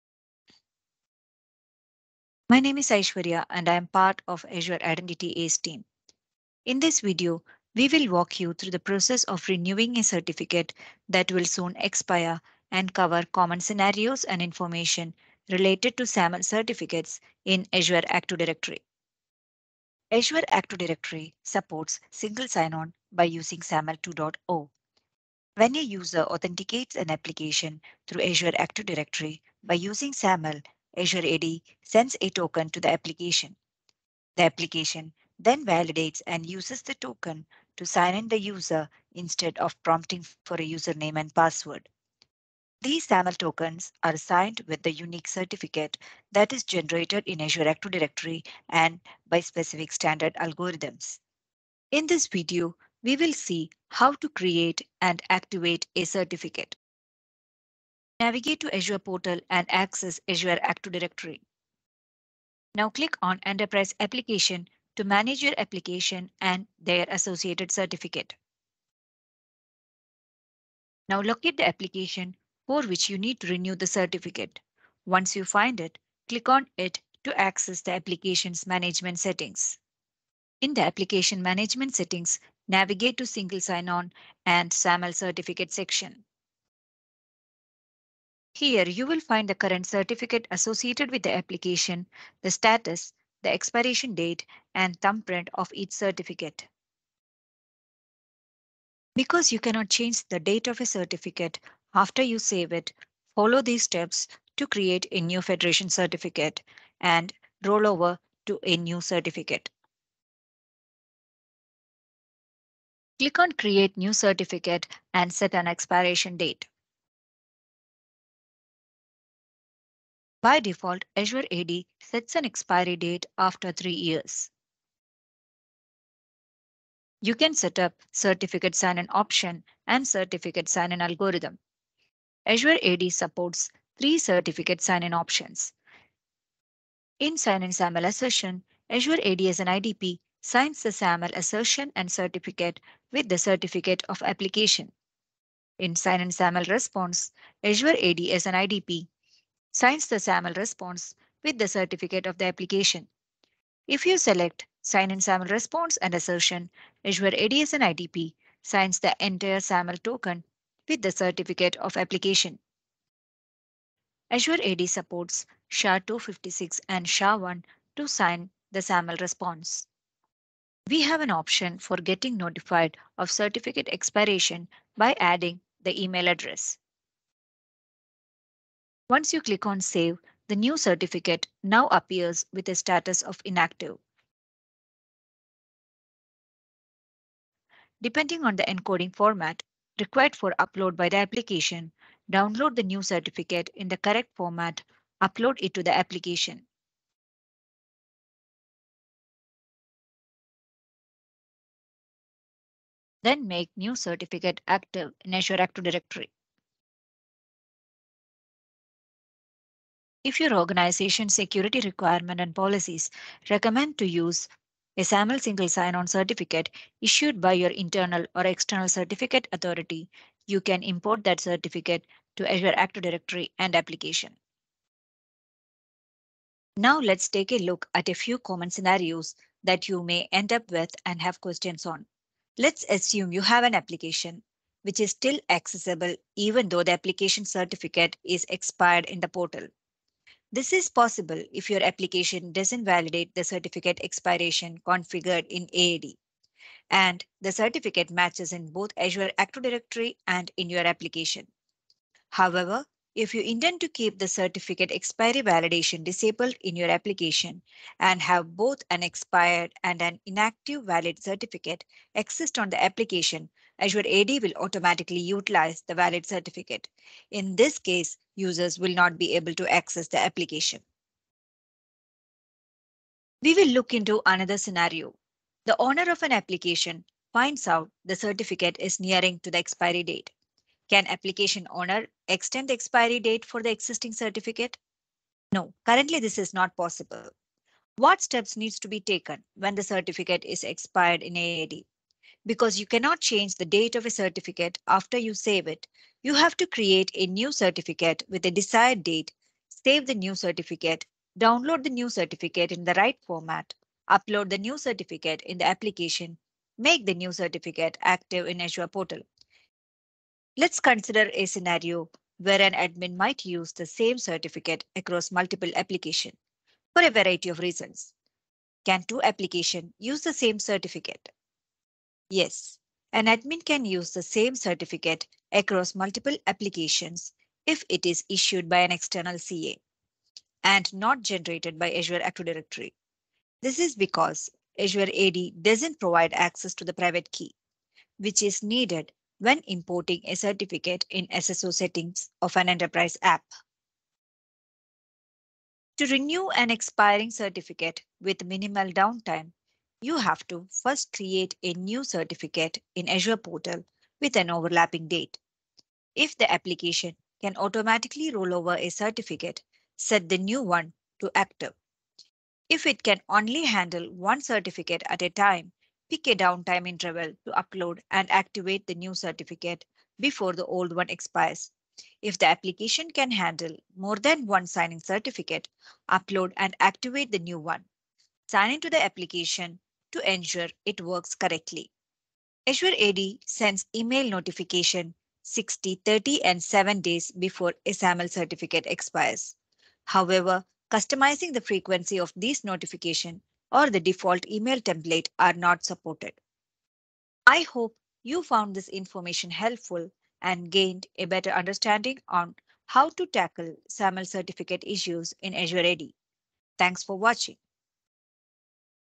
Overview of certificate management for Applications on Microsoft Entra
In this video tutorial from Microsoft, you will receive an overview of the common questions and information related to certificates that Microsoft Entra creates for a SAML application such as how to customize the expiration dates for certificates, add email notification address for certificate expiration dates, and generate and renew certificate